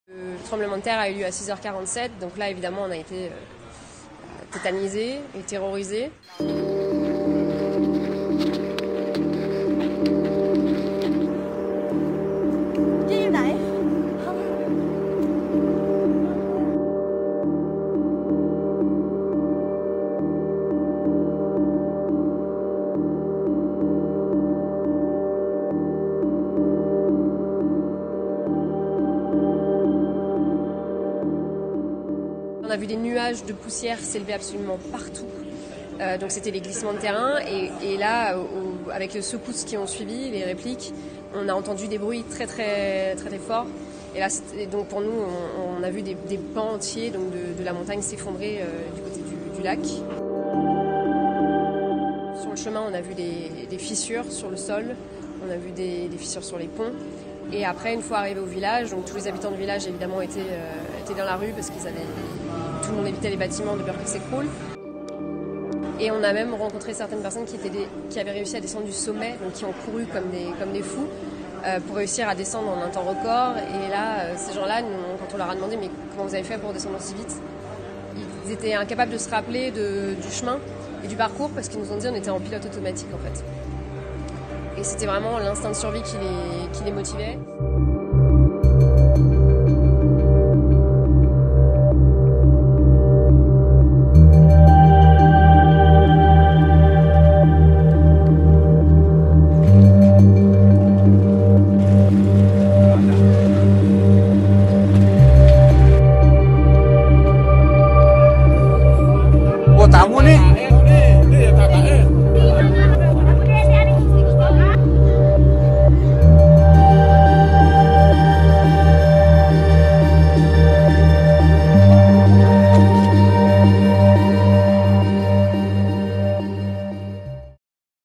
Séisme en Indonésie : une Française témoigne
Un puissant séisme a touché l’île indonésienne de Lombok en Indonésie, le 29 juillet. La magnitude du tremblement de terre était de 6,4. La journaliste Camille Gicquel se trouvait parmi les nombreux randonneurs qui faisaient l’ascension du volcan Rinjani. Elle raconte dans une vidéo envoyée au Monde ce puissant tremblement de terre. « Des morceaux entiers de la montagne tombaient », témoigne-t-elle.

Au moins seize personnes ont été tuées et 335 blessées. Lundi 30 juillet, plus de 500 randonneurs étaient toujours bloqués sur les flans du mont Rinjani, un volcan.